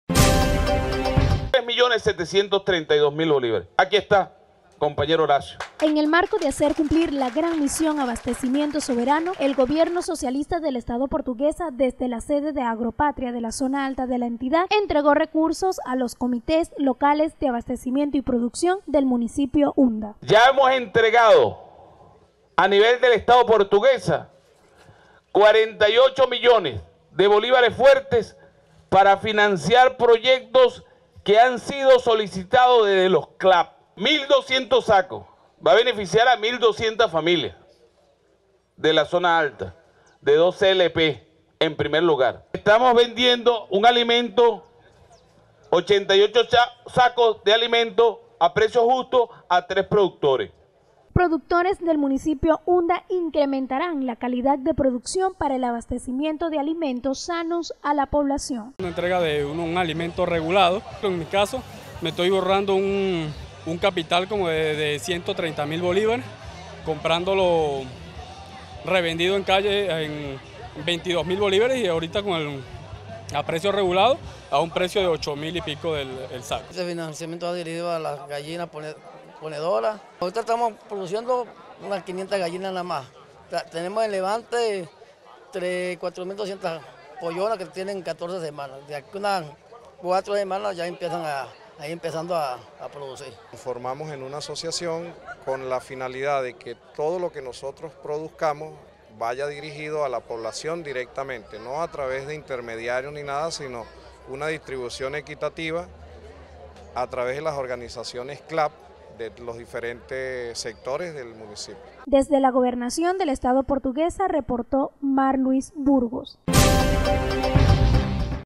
Gobierno del estado Portuguesa entregó financiamientos a productores del municipio Unda
En el marco de hacer cumplir la Gran Misión Abastecimiento Soberano, el Gobierno Socialista del estado Portuguesa desde la sede de Agro Patria de la zona alta de la entidad, entregó recursos a los Comités Locales de Abastecimiento y Producción del municipio Unda